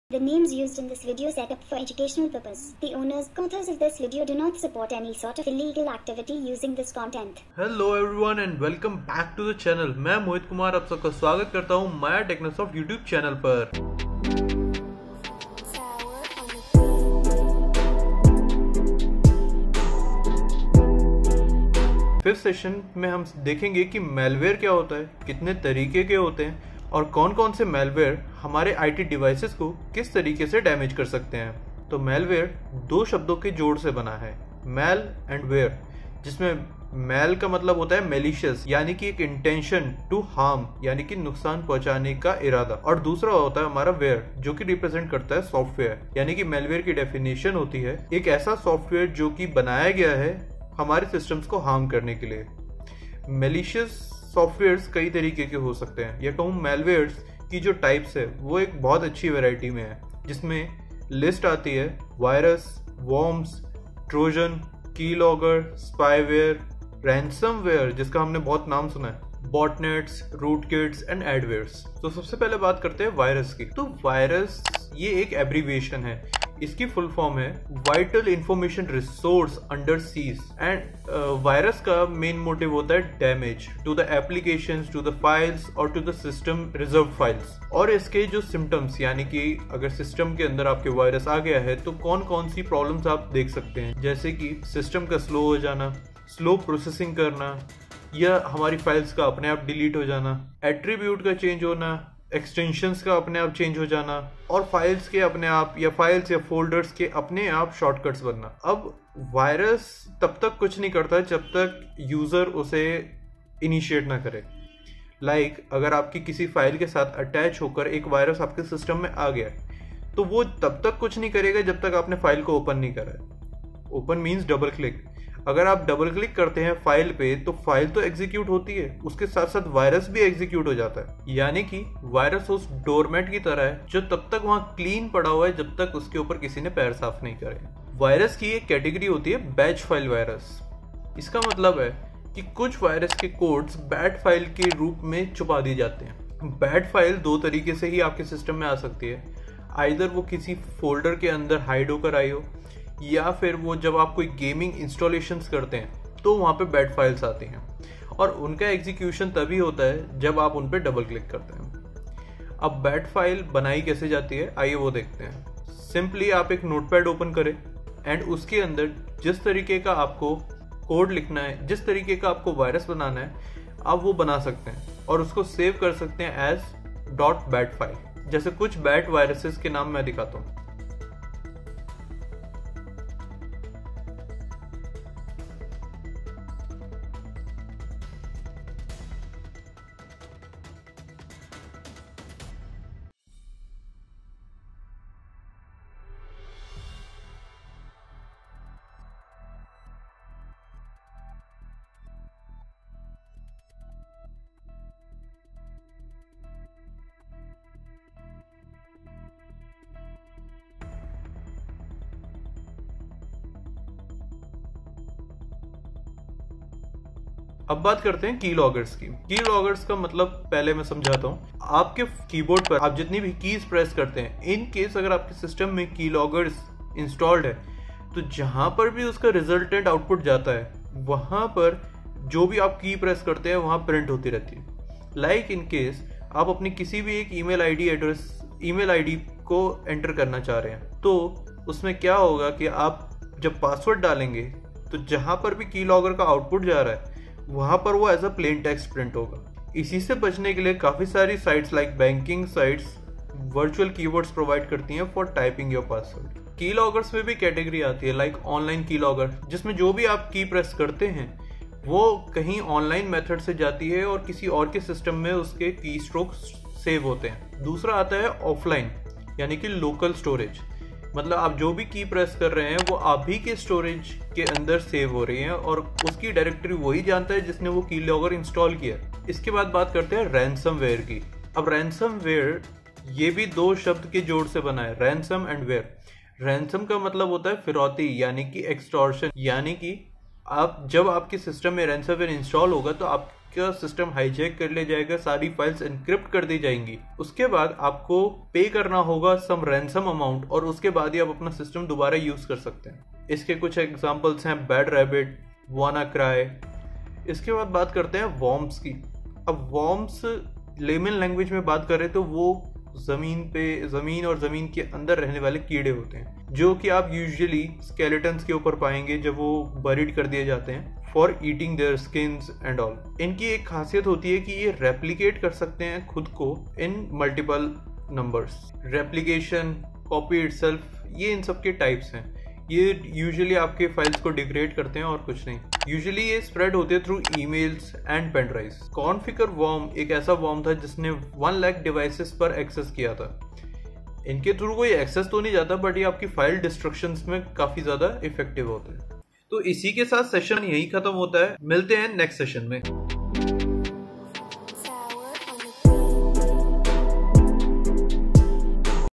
Session-5 Malware | Ethical Hacking Course 2020 Hindi | Maya Techno Soft
Hello viewers,
This is the start of to the new ethical hacking course 2020 in Hindi language with complete explanation and meaning of each term.
Every Monday, there will be a new session and doubt clearance.

Presenter & Editor:
MOHIT KUMAR

In association with MAYA TECHNO SOFT